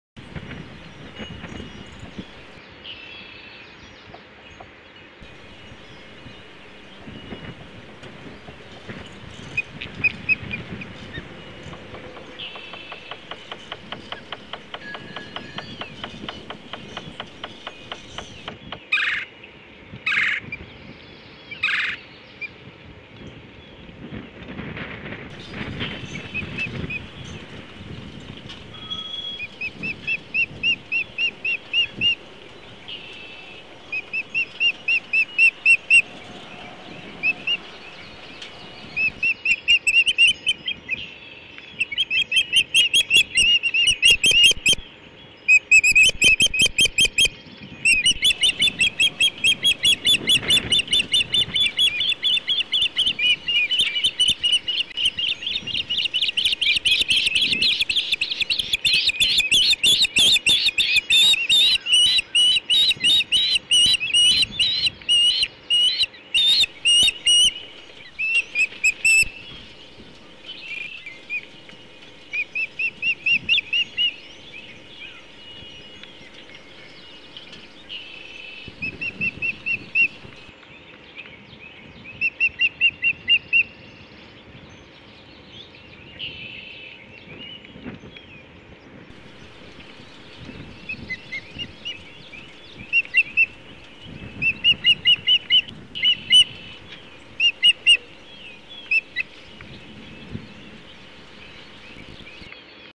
Pine cone placement 13 41 Video 2017 04 08 152616
Mrs O brings in a pine cone & gently places it on the nest. Thanks to Cam Host (Skidaway Audubon) and Cornell Lab of Ornithology for the video feed.  4/8/17  13:41